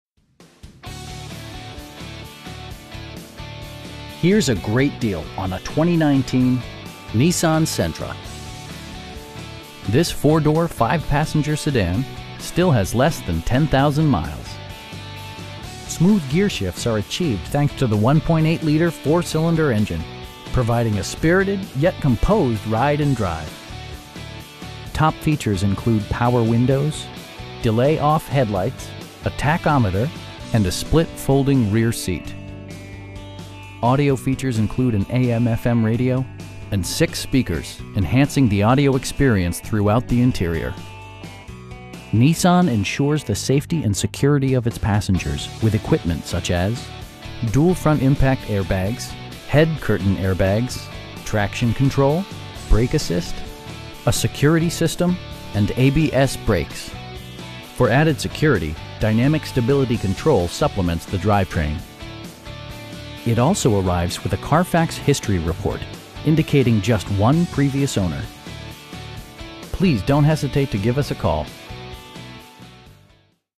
2019 Nissan Sentra SV in Naperville, IL 60540
Here's a great deal on a 2019 Nissan Sentra. This 4 door, 5 passenger sedan still has less than 10,000 miles! Smooth gearshifts are achieved thanks to the 1.8 liter 4 cylinder engine, providing a spirited, yet composed ride and drive. Top features include power windows, delay-off headlights, a tachometer, front bucket seats, tilt steering wheel, power door mirrors, an overhead console, and a split folding rear seat. Audio features include an AM/FM radio, and 6 speakers, enhancing the audio experience throughout the interior. Nissan ensures the safety and security of its passengers with equipment such as: dual front impact airbags, head curtain airbags, traction control, brake assist, a security system, and ABS brakes. For added security, dynamic Stability Control supplements the drivetrain. It also arrives with a Carfax history report, indicating just one previous owner. We know that you have high expectations, and we enjoy the challenge of meeting and exceeding them! Please don't hesitate to give us a call.